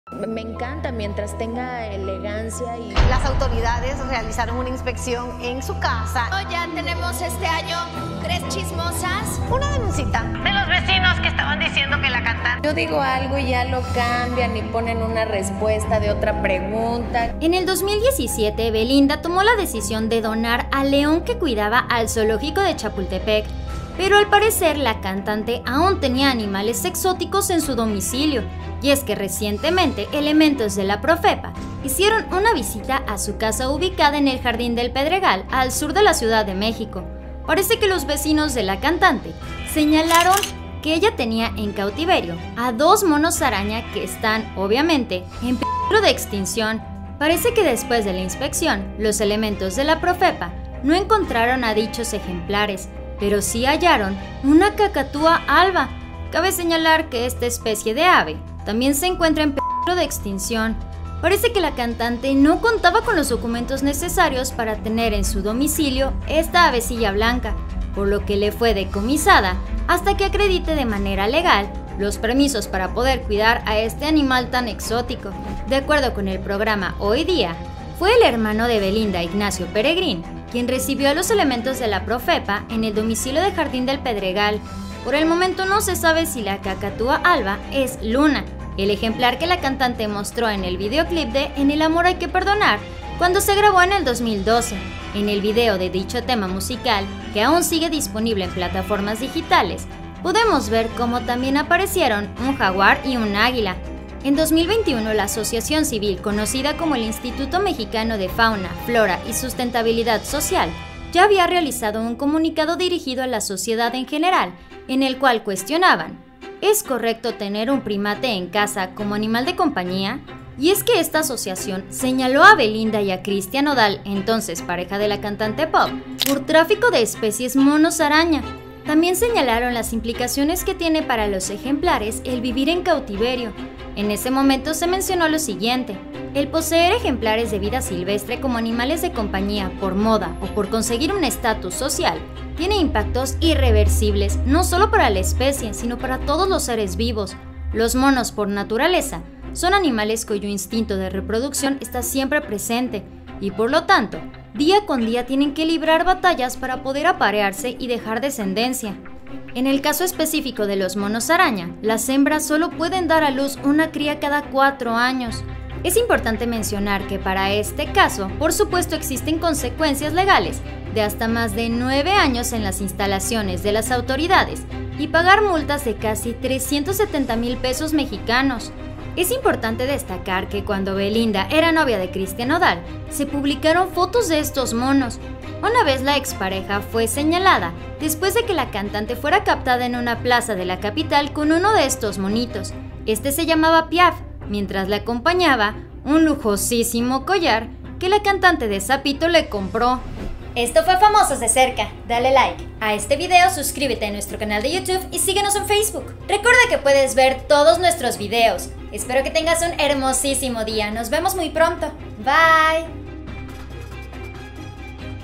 😱 ¡ 𝗟𝗘 𝗖𝗔𝗬𝗢 𝗟𝗔 𝗟𝗘𝗬 ! Confiscarón La Casa de Belinda Por Tener Animales 𝗘𝗫𝗢𝗧𝗜𝗖𝗢𝗦 🔥
¡ 𝗟𝗘 𝗖𝗔𝗬𝗢 𝗟𝗔 𝗟𝗘𝗬 ! Confiscaron La Casa de Belinda Por Tener Animales 𝗘𝗫𝗢𝗧𝗜𝗖𝗢𝗦

Le Llego el Karma Belinda Tendra que Pagar Caro por tener animales en peligro de extincion.

 BIENVENIDOS!!!
suelta la sopa
televisa
exclusivas
venga la alegria
tv azteca
polemica
chismes
Entertainment
farandula mexico